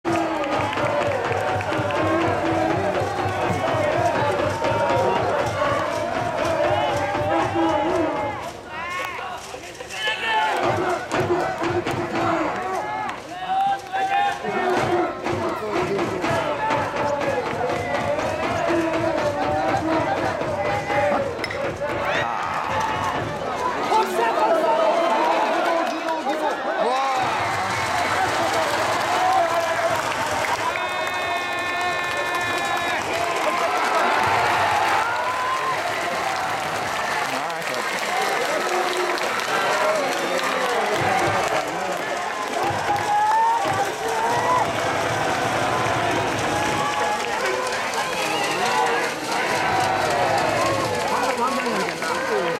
久留米商業高 泉 大輝 選手 右中間タイムリースリーベース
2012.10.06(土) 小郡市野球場
第131回九州地区高校野球福岡県大会一回戦
久留米商業高校 対 東福岡高校